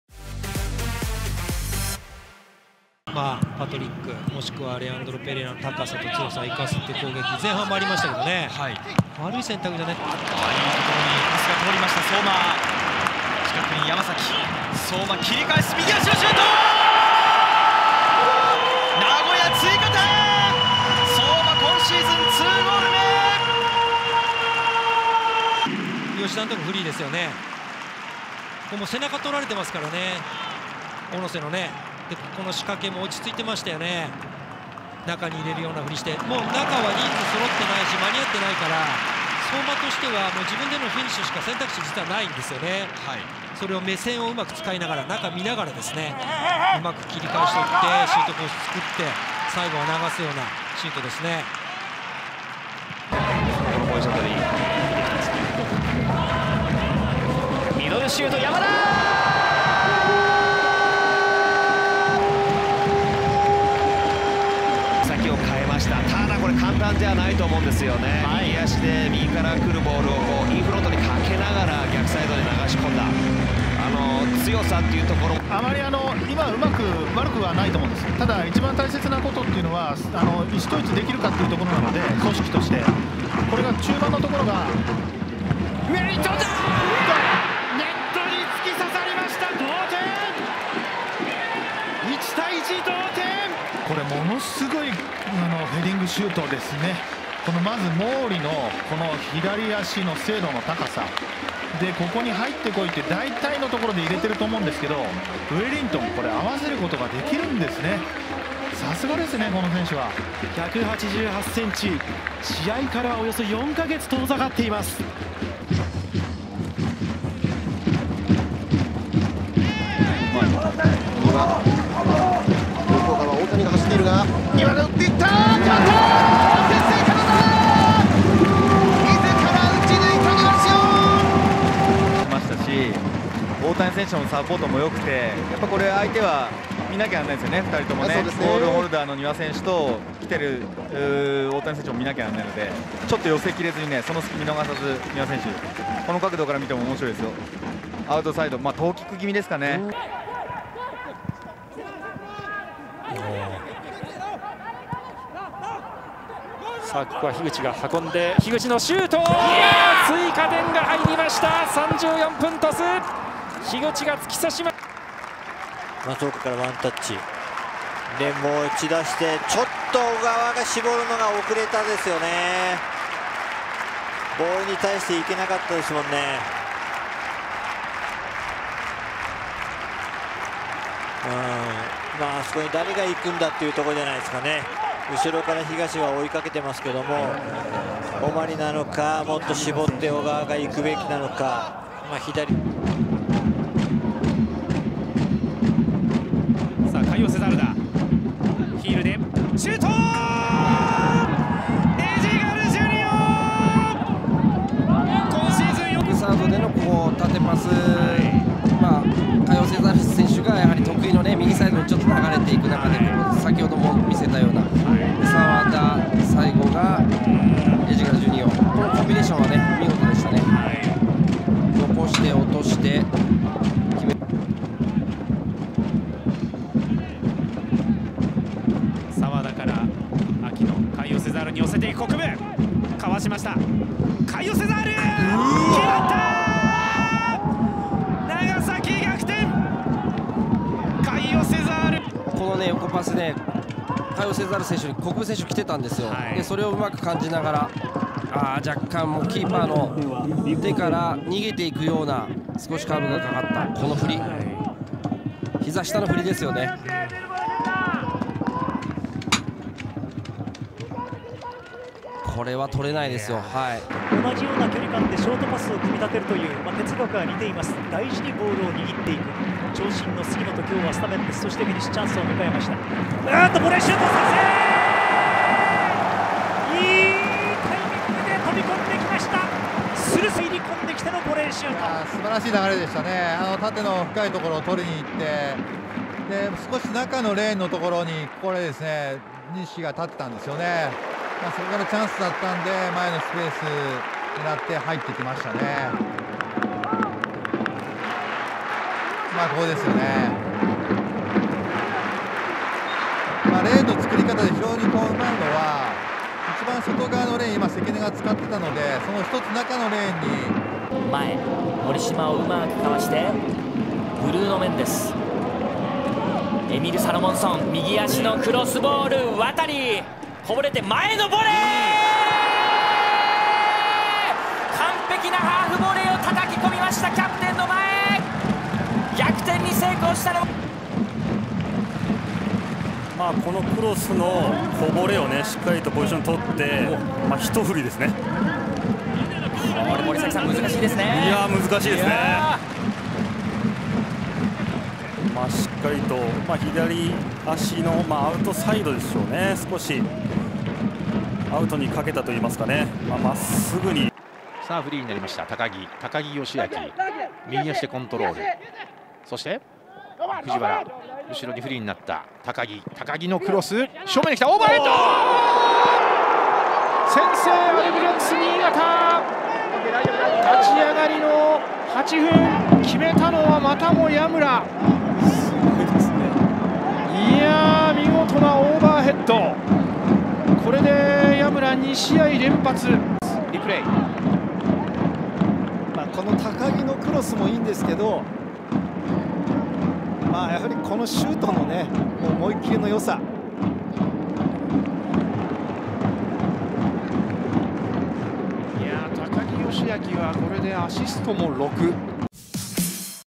Top 10 Goals of the Week | 21st-25th April | 2021 MEIJI YASUDA J.LEAGUE
These are your top 10 goals from 21st to 25th April of the 2021 MEIJI YASUDA J.LEAGUE.
Whose goal will make the list? Find out right here!